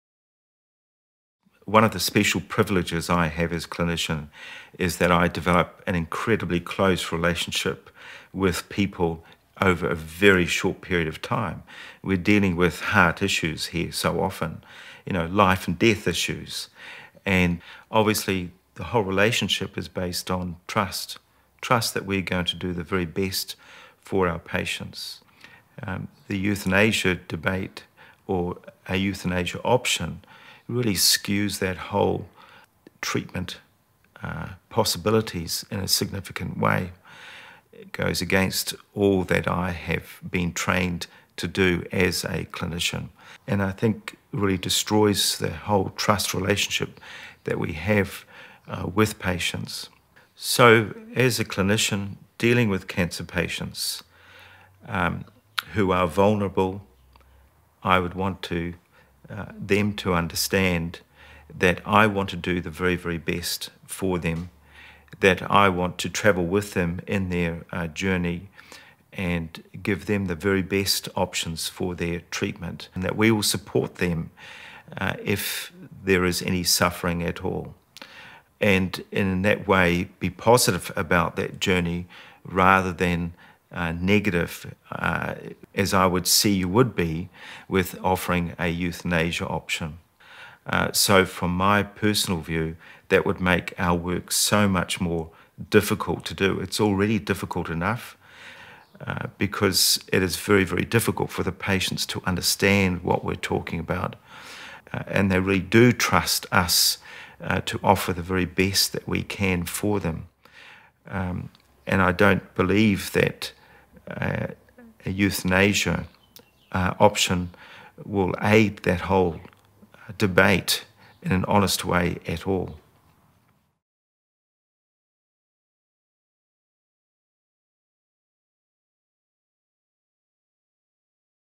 16,000 Voices (Ralph's story)
Ralph shares his experience and perspective as cancer doctor. Ralph is one of the 16,000 Voices who said no, we don't want euthanasia in New Zealand.